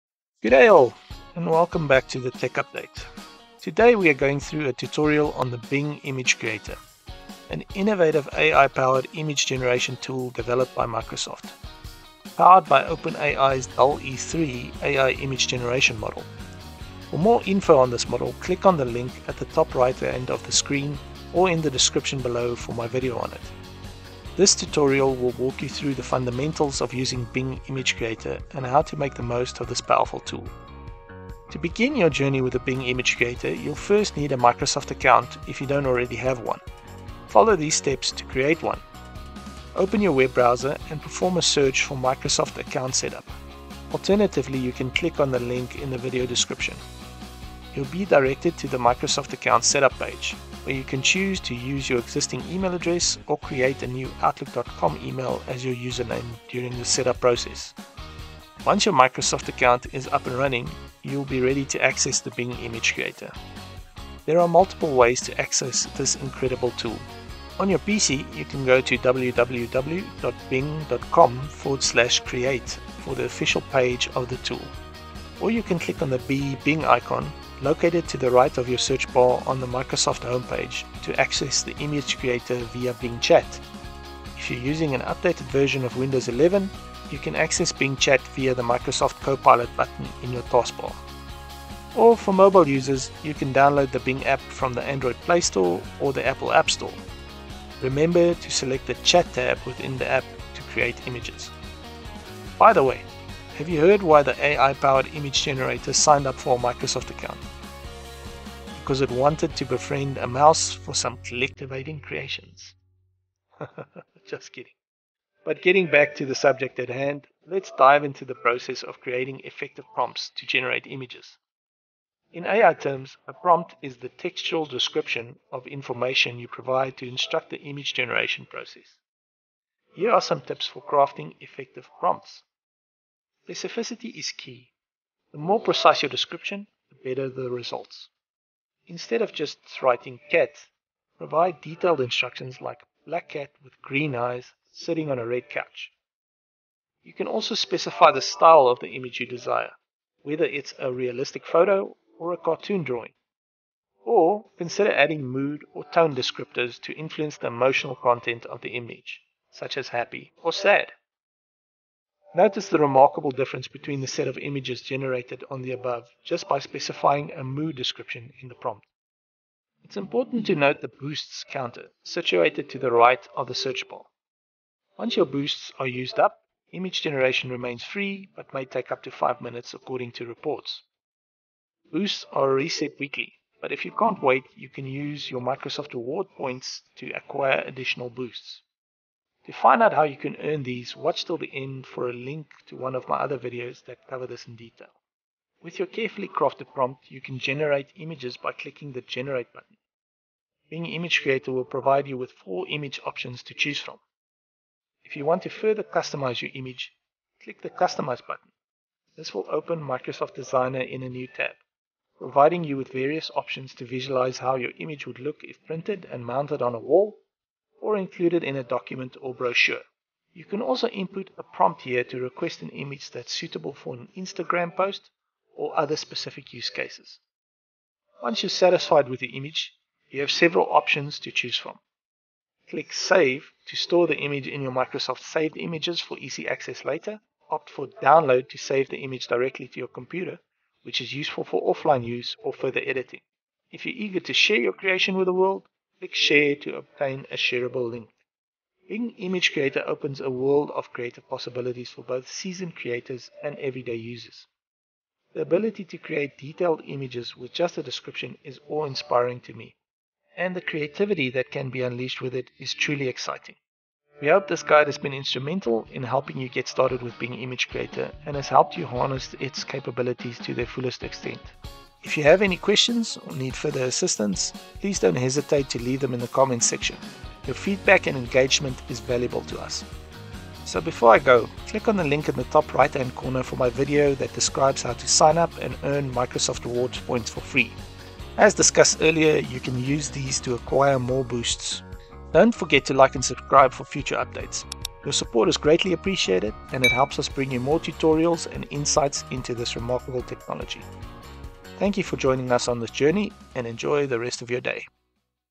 Bing Image Creator: A Tutorial for Beginners
Discover the boundless creative potential of Bing Image Creator in our comprehensive tutorial! Unleash your imagination with step-by-step guidance on creating vivid, AI-generated images using Microsoft's cutting-edge tool. Learn how to craft effective prompts for jaw-dropping results, from playful cats to serene landscapes. Boost your image generation prowess with insider tips and insights. Plus, find out how to keep those creative juices flowing with Microsoft Rewards points. Join us for an exciting journey into the world of AI artistry. Your creative adventures start here!